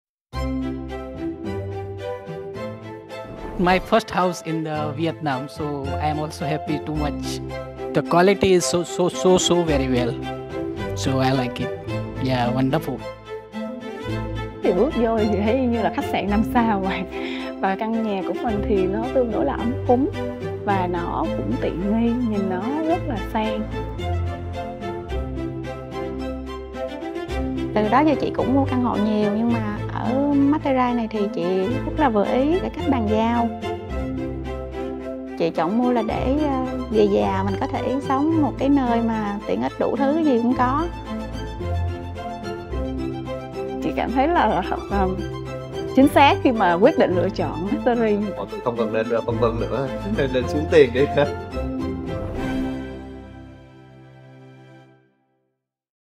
LUMIÈRE Boulevard | CHẤT LƯỢNG CĂN HỘ THUYẾT PHỤC CƯ DÂN VỀ SINH SỐNG
Cuộc sống Xanh và Sang tại LUMIÈRE Boulevard quá “cuốn”.

Đó là lý do vì sao nhiều khách hàng đầu tư đổi ý muốn về đây ở và trải nghiệm. Hãy cùng lắng nghe chia sẻ của chính những khách hàng này về sức hút của cuộc sống tại Lumière Boulevard.